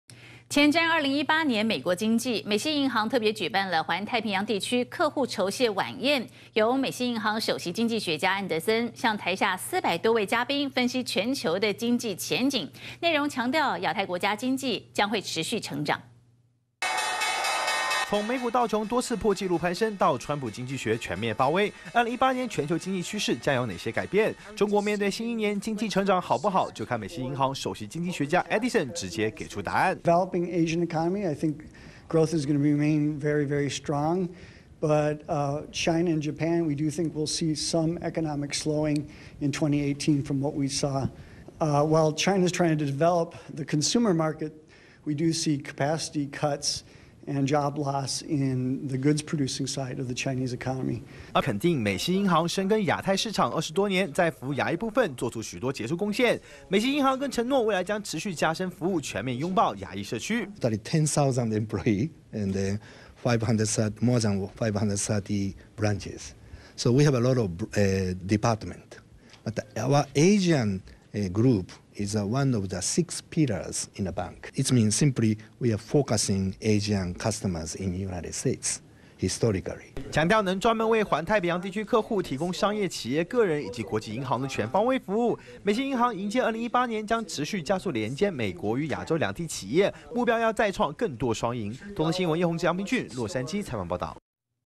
美西銀行預測 2018年經濟持續成長
前瞻2018年美國經濟! 美西銀行，特別舉辦「環太平洋地區 客戶酬謝晚宴」。由「美西銀行首席 經濟學家」，安德森，向台下四百多位嘉賓，分析全球經濟前景。內容強調，亞太國家經濟，將持續成長。